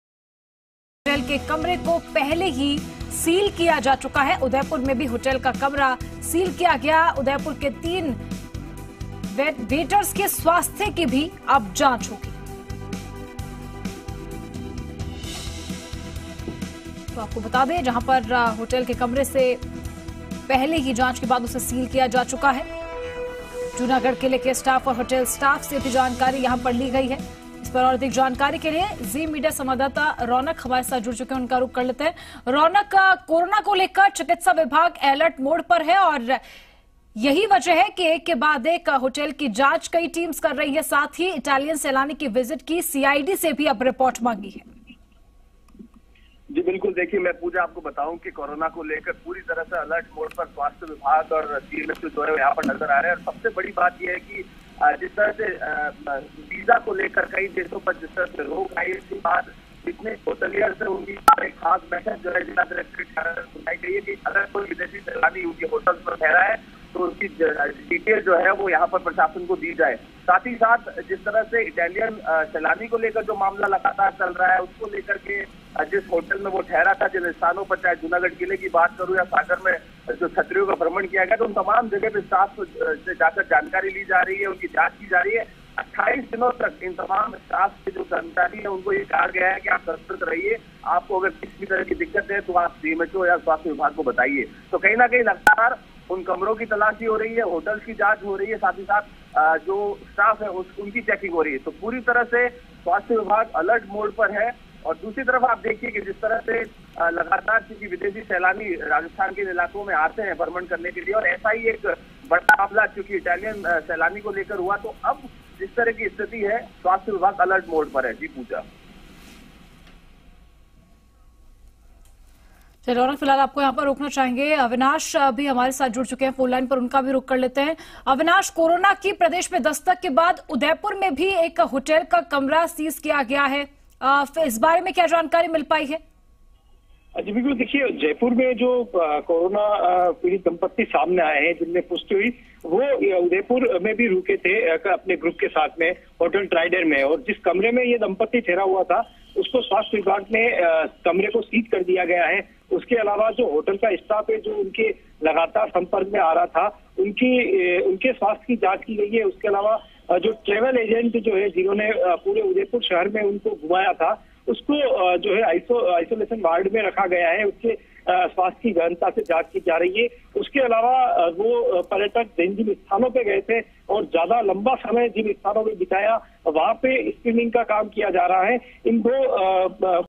Corona virus Reaches Bikaner | होटल की गई जांच, राजस्थान में भी अलर्ट
जयपुर - कोरोना वायरस (Corona virus ) को लेकर राजस्थान में भी अलर्ट... सभी चिकित्सा कर्मियों की छुट्टियां हुईं कैंसिल... जयपुर के SMS अस्पताल में भर्ती विदेशी दंपति भी कोरोना वायरस पॉजिटिव..उदयपुर भी गया था कोरोना पॉजिटिव विदेशी पर्यटक... इसके साथ ही पर्यटक बीकानेर भी गया था जहां उसके ठहरने वाले होटल में जांच की गई।